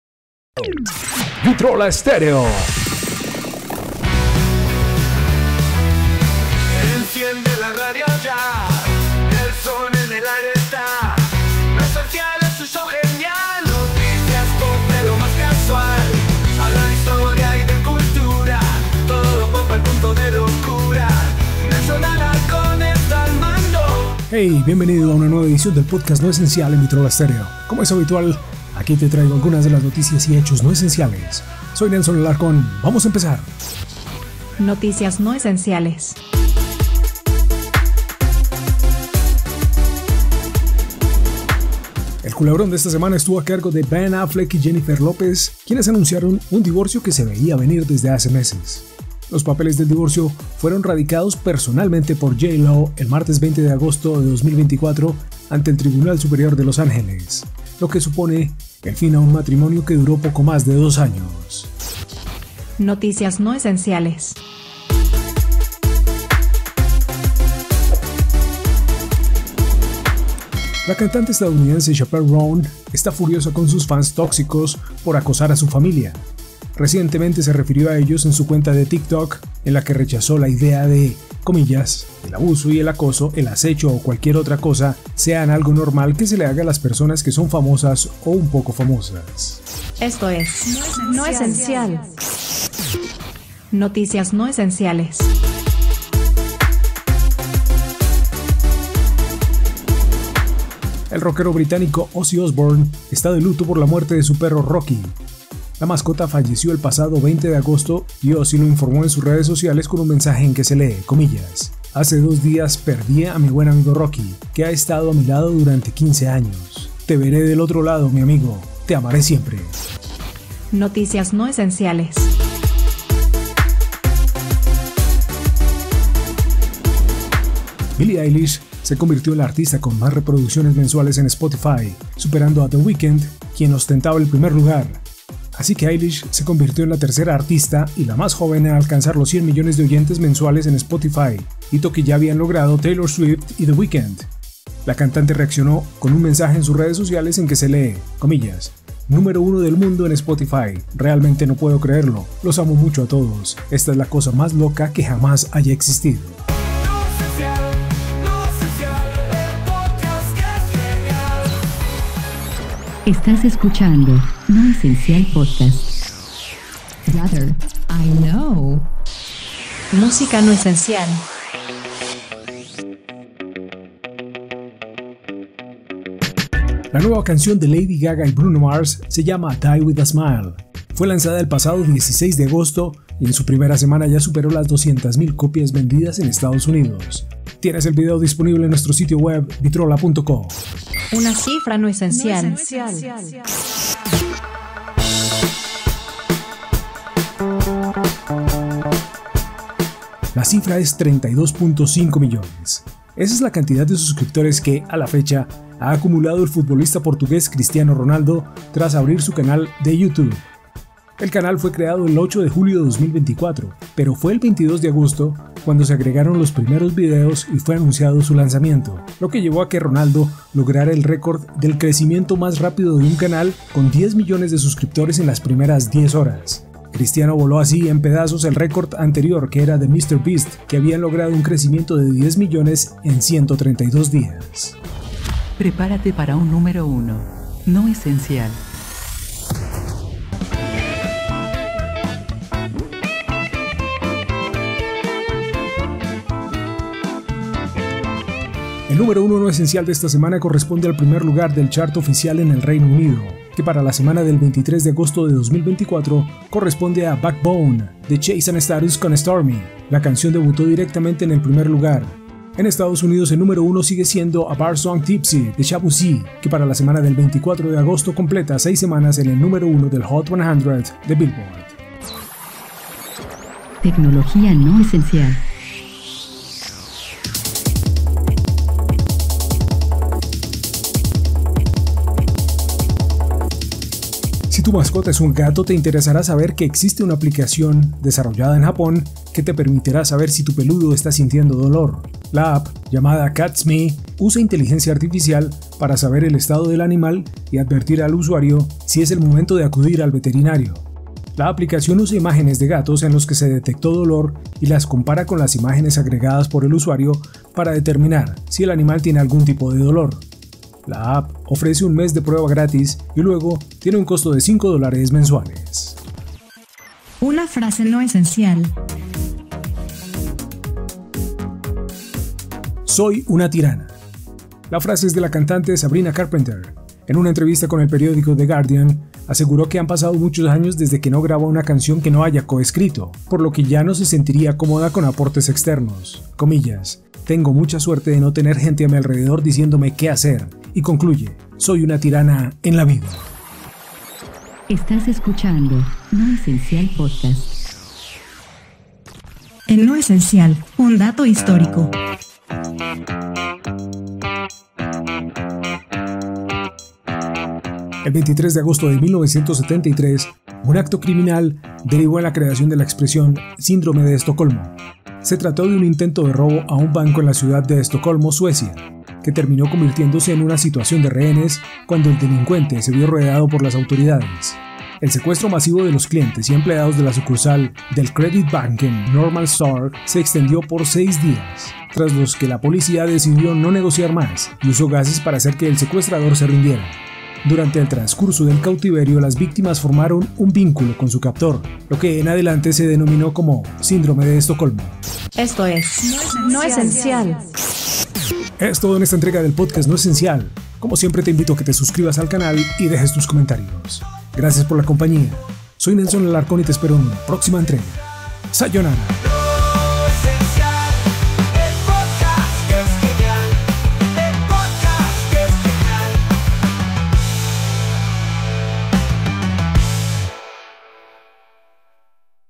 ▶ Cristiano Ronaldo rompe records en Youtube 🎤 Podcast NO ESENCIAL #94
En esta edición del podcast No Esencial hablamos del record alcanzado por Cristiano Ronaldo en su debut como youtuber y otras noticias de la semana.

▶ Cristiano Ronaldo rompe records en Youtube 🎤 Podcast NO ESENCIAL #94 con Nelson Alarcón | @VitrolaStereo

Voiceover: @alarconnelson

Vitrola Stereo, Classics from the Rock era!